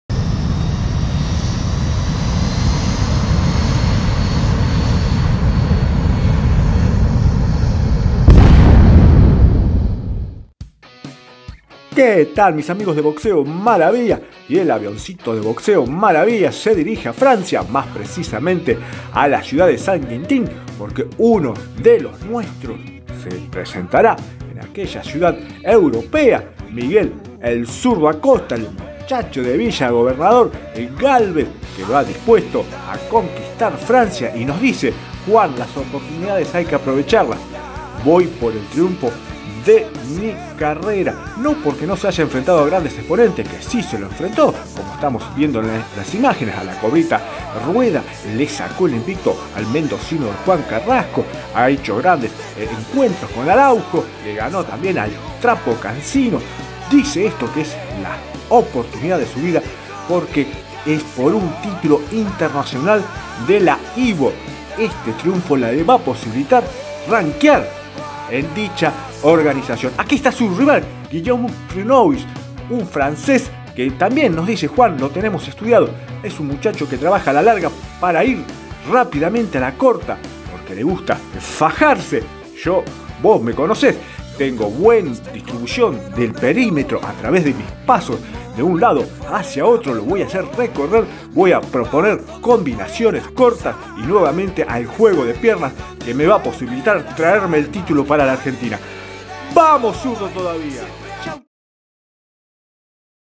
Guillaume Frenois vs Miguel Acosta. Promo de la pelea. Información
Miguel Acosta en Francia
El muchacho de Villa Gobernador Gálvez se presenta en Saint-Quentin en donde va, tal vez, por la victoria mas importante de su carrera. Su rival será el local de Guillaume Frenois y la importancia de este match radica en el hecho de que va por el Título Internacional Ligero de la IBO. Miércoles 6 de Julio. Ampliaremos